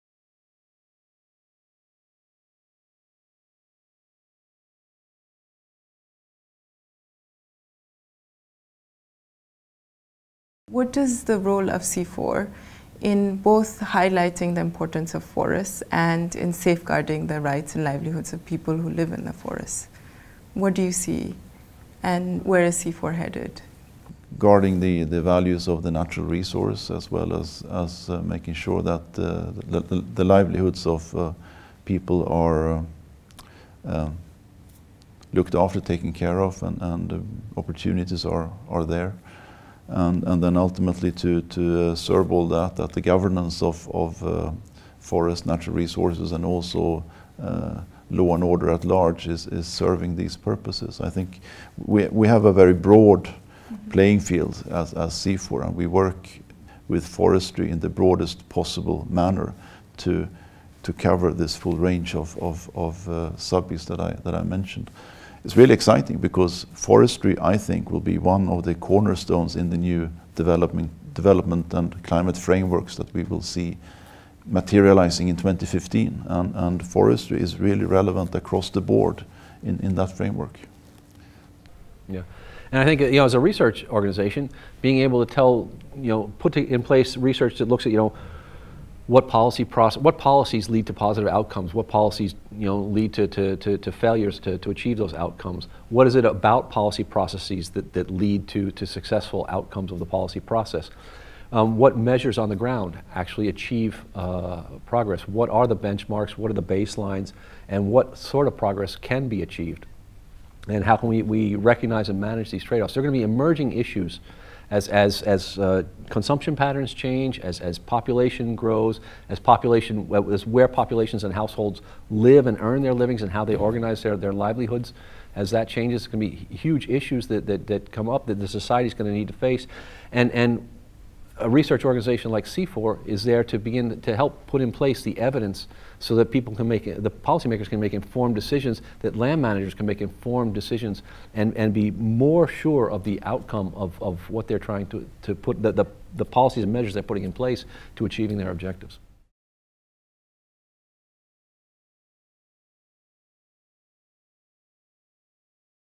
Forests: how CIFOR keeps them on the agenda
The role of CIFOR is making sure that when climate change is discussed, forests are always on the agenda.